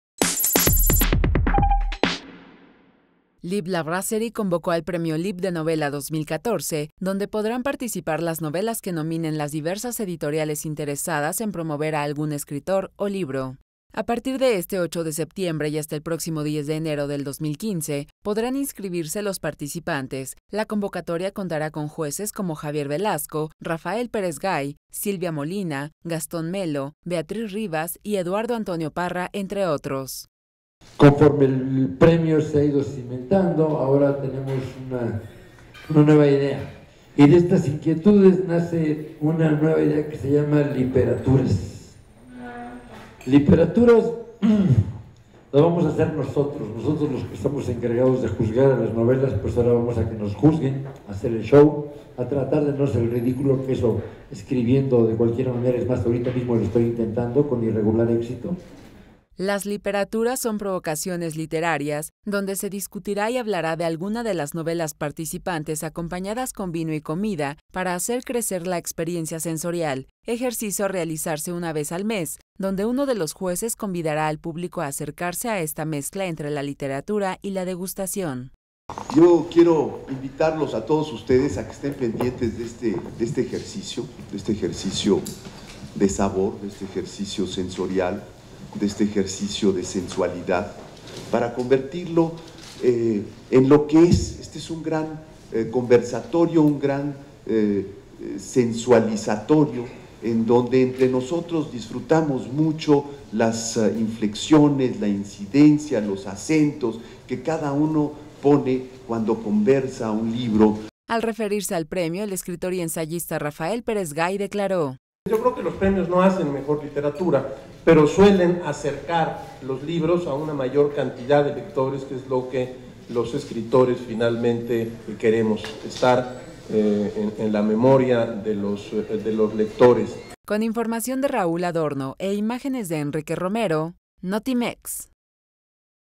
Lanzan convocatoria Premio Literario Lipp La brasserie
Lipp La brasserie convocó al premio Lipp de Novela 2014, donde podrán participar las novelas que nominen las diversas editoriales interesadas en promover a algún escritor o libro.